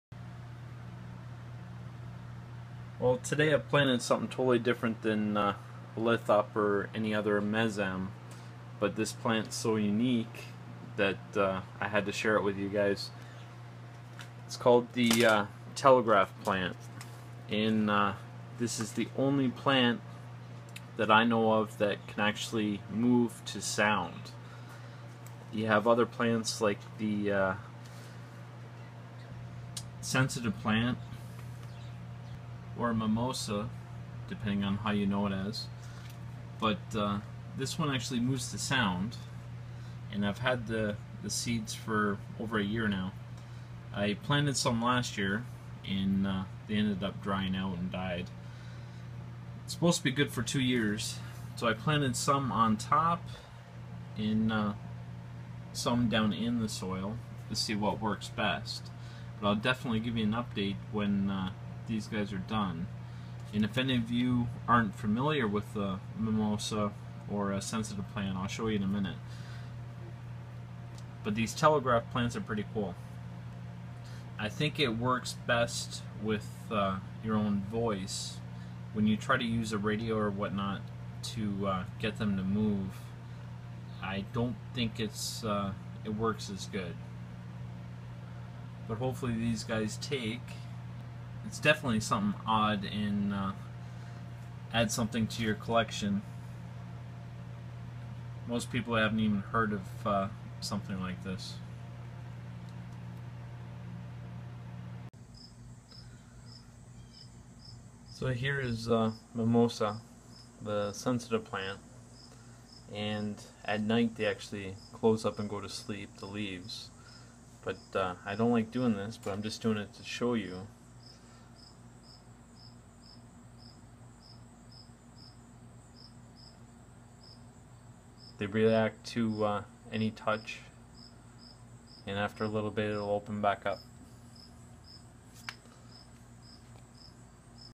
Planted Telegraph Plant Seeds (Dancing Plant) Moves To Sound
The telegraph plant is the only plant that moves to sound / music that I know of.   Scientists don't know why this plant has developed this ability and they don't see any advantages in this for the plant.
If anyone has any advice it would be much appreciated